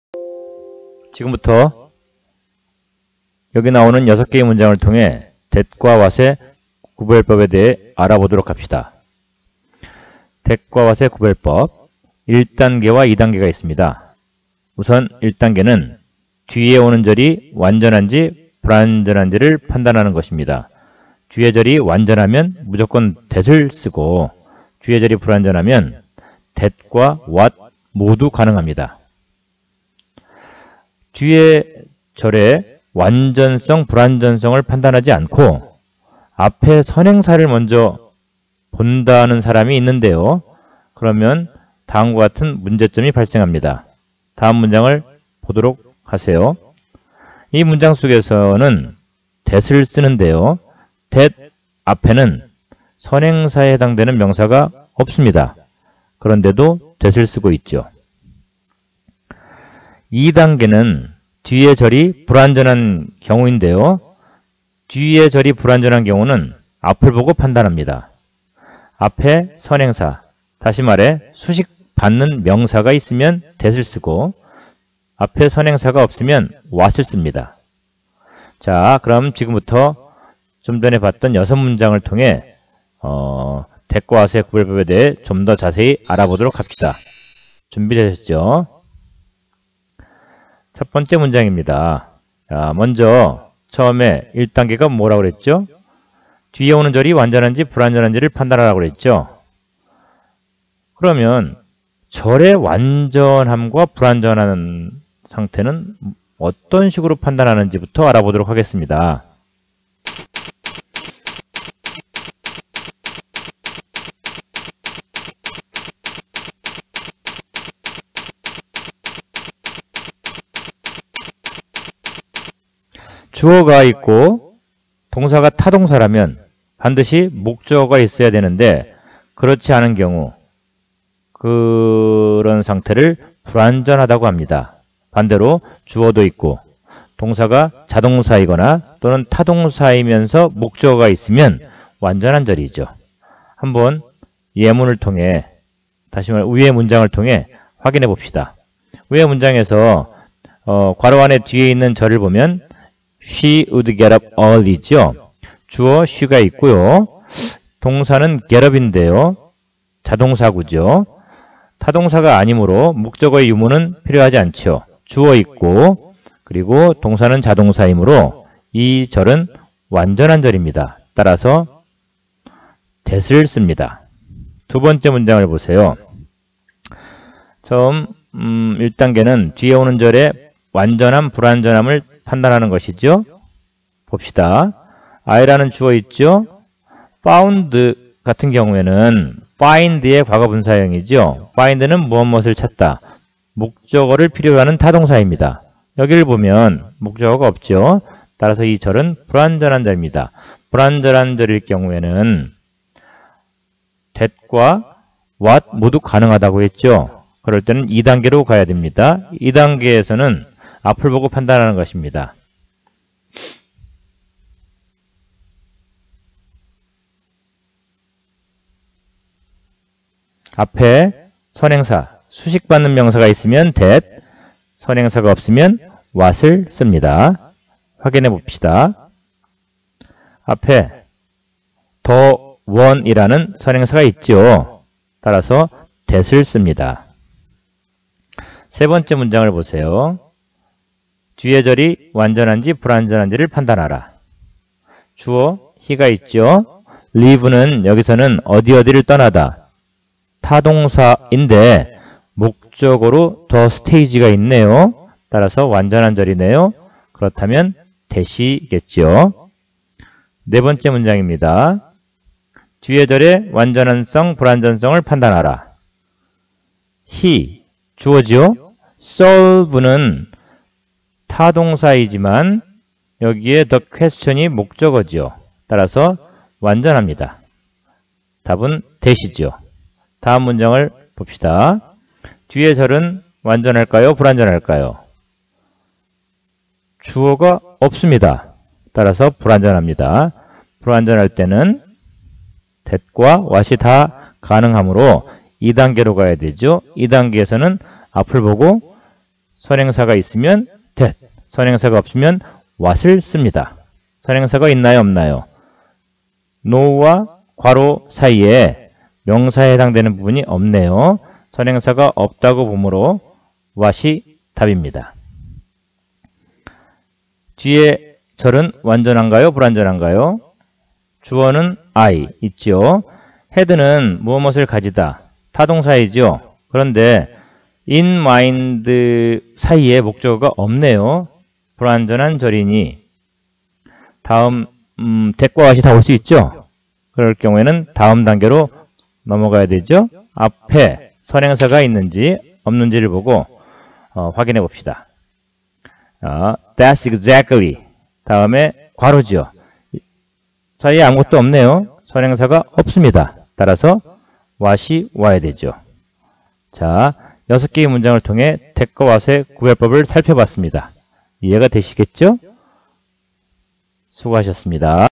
that과 what의 구별법
수능어법과 내신에서 가장 많이 다루고 있는 문법 항목인 that과 what의 구별법에 대해 정리했습니다.